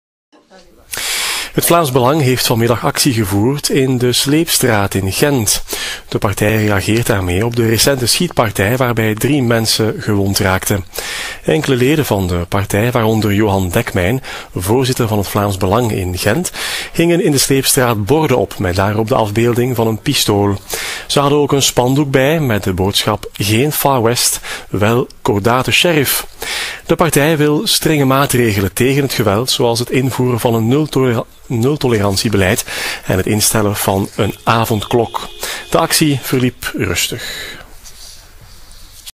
Vlaams Belang voert actie in Gentse Sleepstraat
AVS-uitzending nav de actie die een delegatie van Vlaams Belang Koepel Gent op 10 december hield ivm de recente schietpartij in de Gentse Sleepstraat.